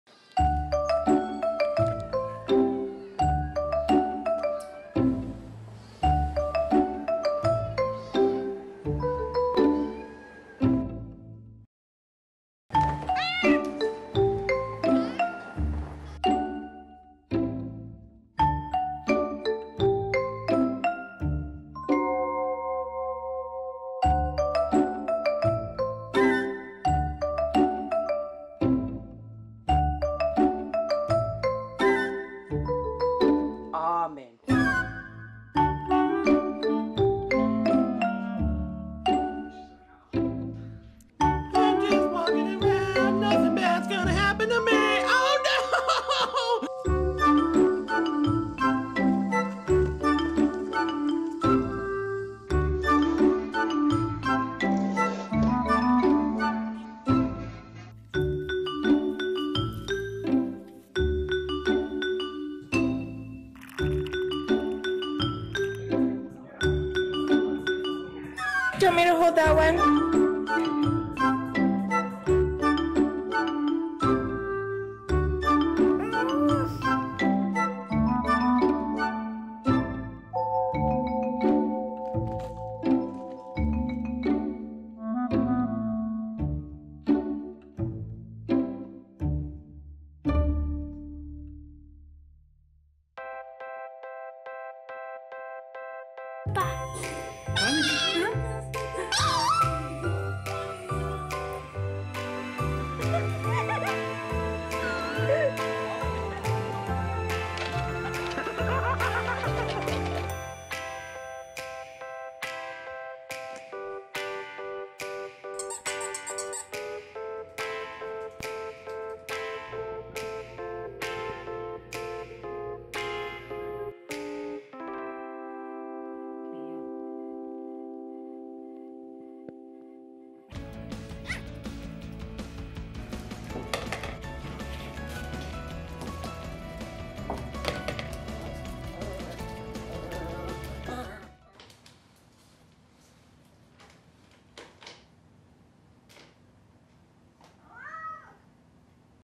Five Phases Of A Food Coma | Thanksgiving Dinner for Cuties
You know how how Thanksgiving dinner normally goes: get really hungry, devour the meal until you're stuffed, sneak a little pie in, then sleep.